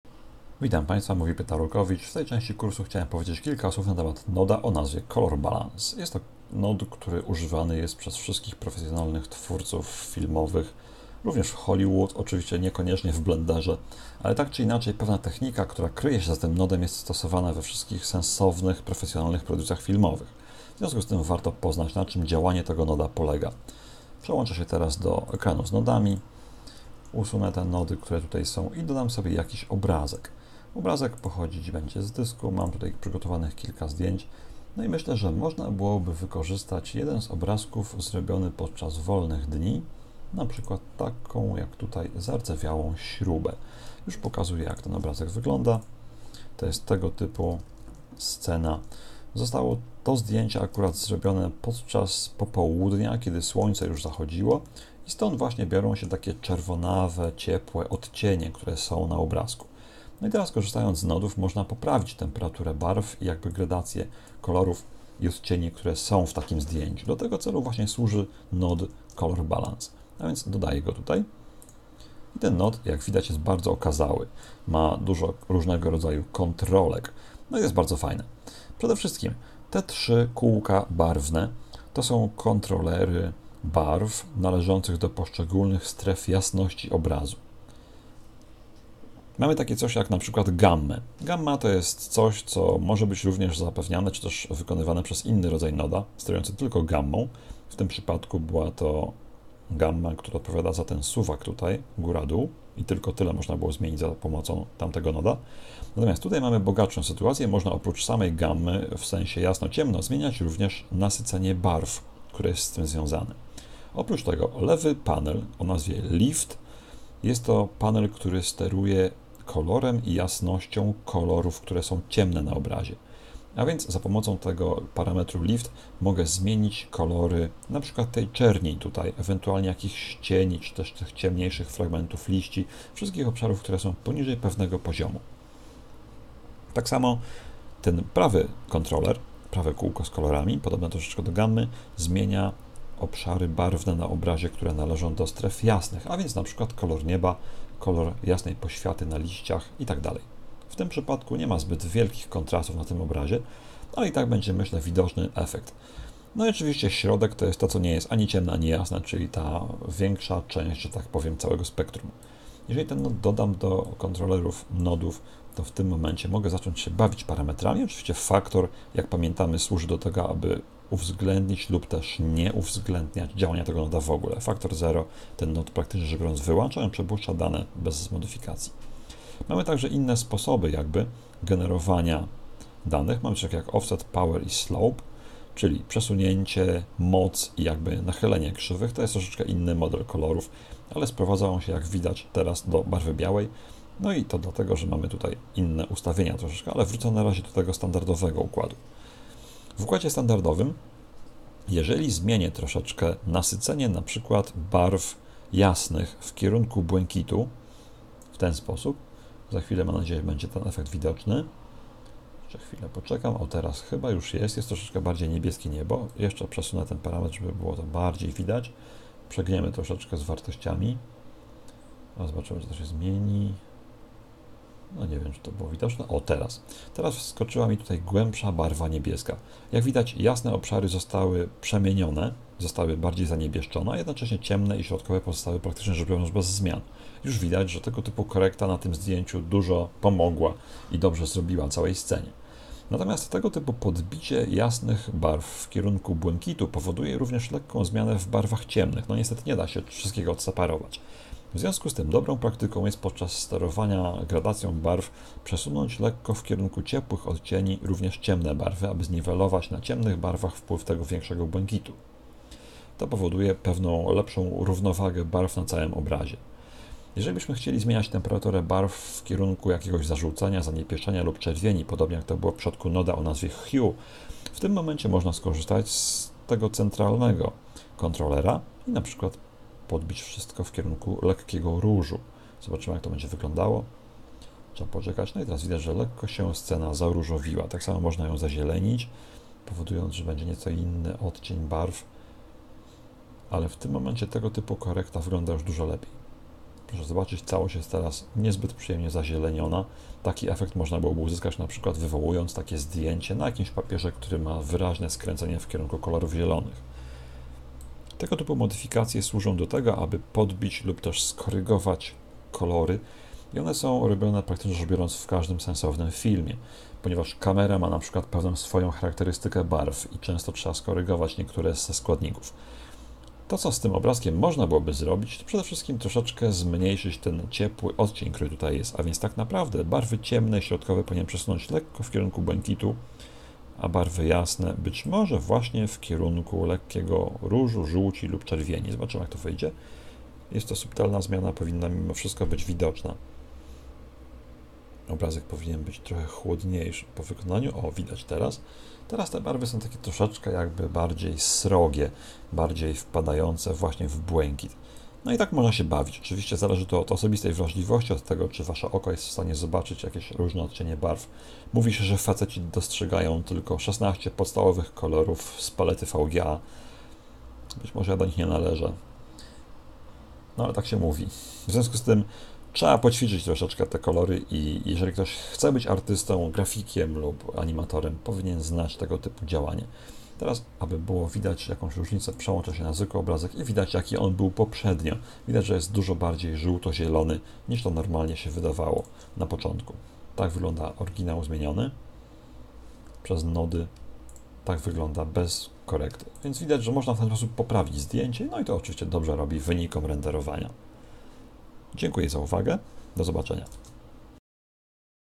Polski Kurs Blendera: Edytor Nodów - Color Balance
Balans kolorów to narzędzie używane w profesjonalnych produkcjach do korekty temperatury barw i odcienia tonalnego całego obrazu.